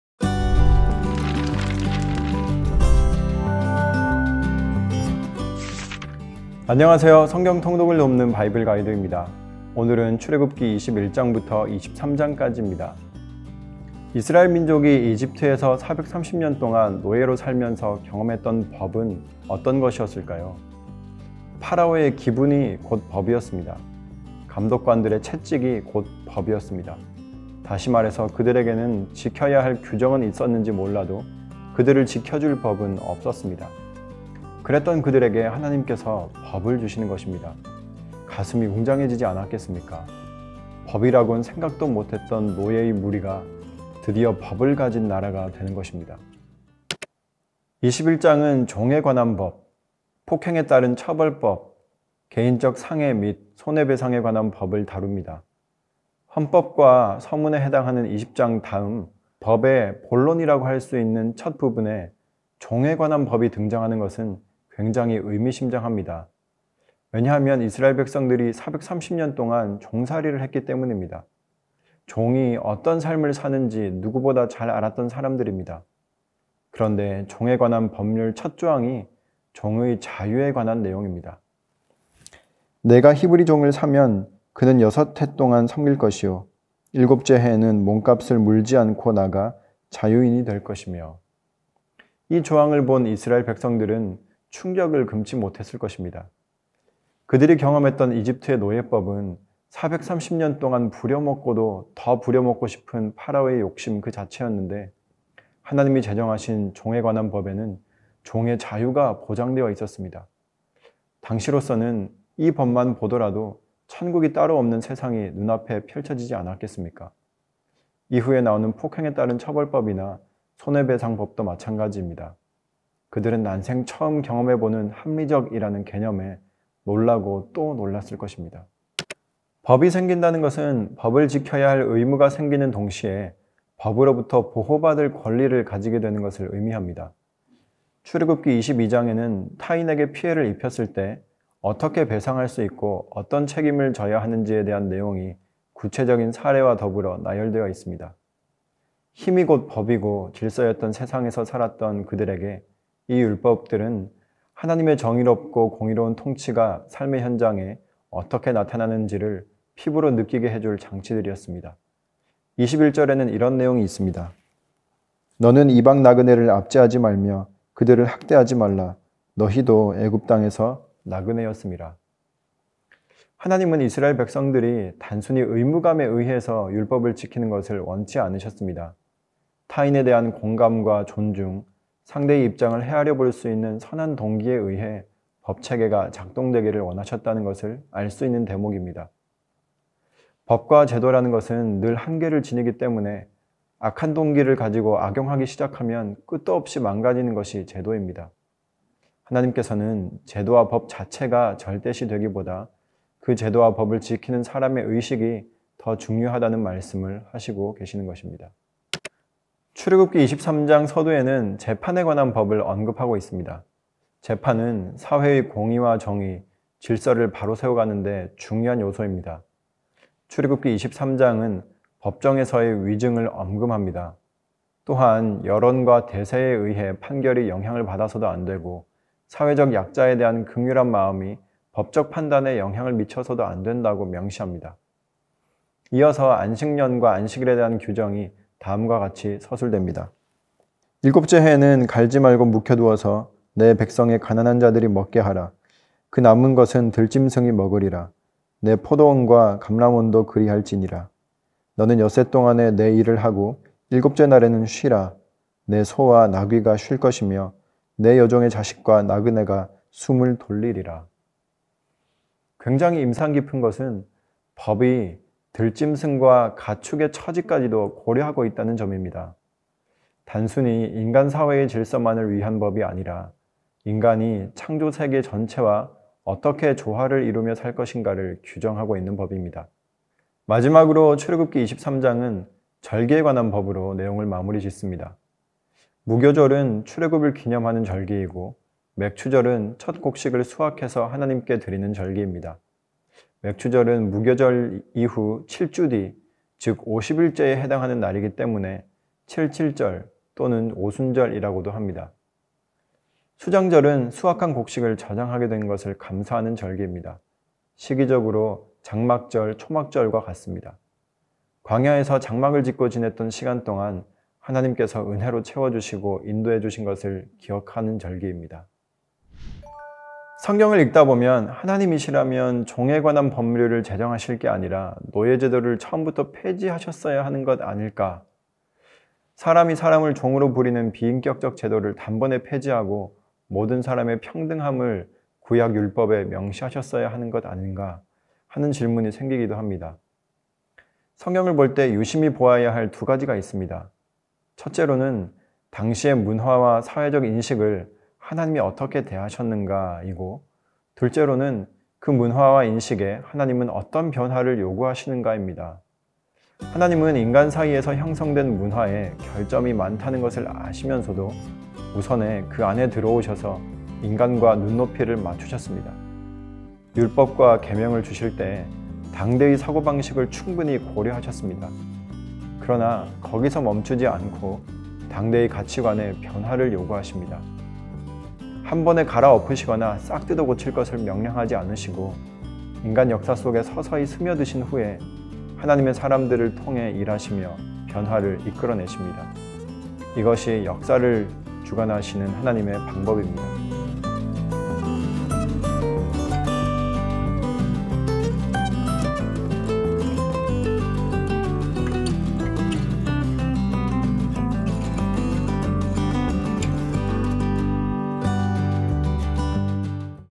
[성경해설] 출애굽기 21-23 | 2024 바이블가이드 Bible Guide _석문섭 목사
[2024 바이블가이드 25강]
매일의 성경 통독을 위한 성경 해설 가이드

진행: 석문섭 목사 (베이직교회)

[구간 빠르게 찾기]
00:00 오프닝
00:34 출애굽기 21장
02:09 출애굽기 22장
03:29 출애굽기 23장